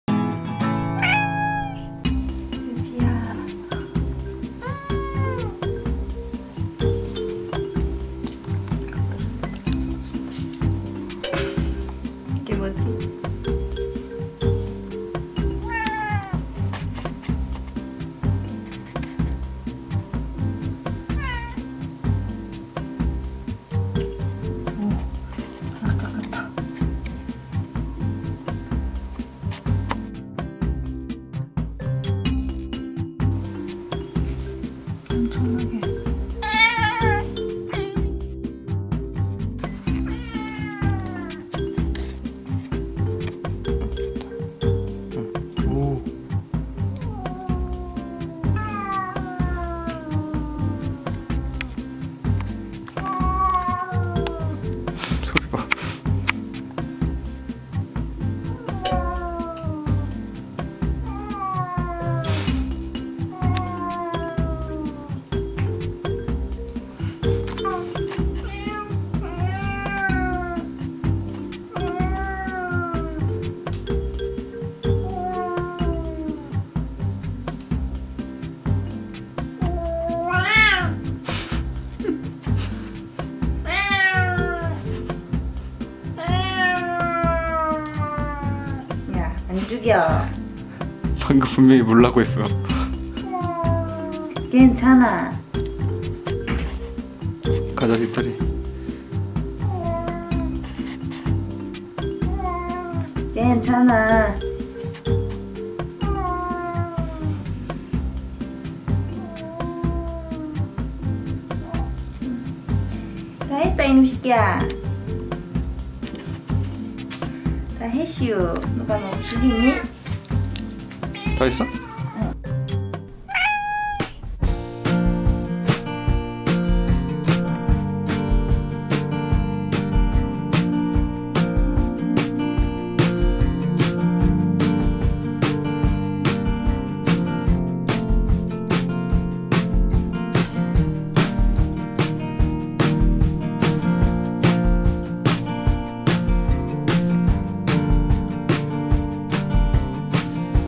엄살 대마왕 휴지의 발톱을 깎아줍니다
ㅋㅋㅋㅋ시끄러운 엄살치고는 반항이 심하지 않아요. 엄살 진짜 세계최강